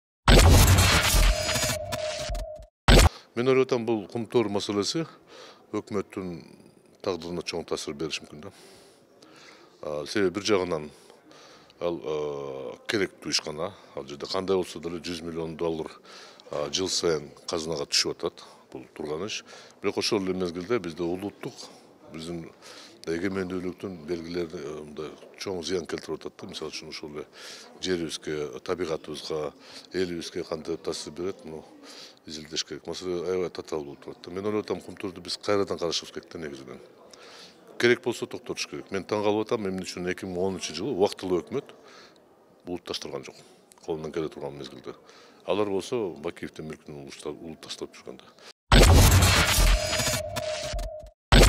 Масалиев: Кумтөр маселеси өкмөттүн тагдырына чоң таасир бериши мүмкүн😲
Кумтөр маселеси өкмөттүн тагдырына чоң таасир бериши мүмкүн. Мындай пикирин Жогорку Кеңештин депутаты Исхак Масалиев айтты.

“Кумтөр бир четинен керектүү ишкана. Ал жерде кандай болсо да 100 миллион доллар жылына казынага түшүп жатат. Бирок, ошол эле мезгилде, биздин эгемендүүлүктүн белгилерине чоң зычн келтирип жатат. маселе аябай татаал болуп турат. Андыктан биз кайрадан карашыбыз керек, эгер керек болсо токтотушубуз керек. Мен таң калып жатам, эмне үчүн 2010-жылы убактылуу өкмөт улутташтырган жок? колунан келип турган мезгилде”,-деди депутат.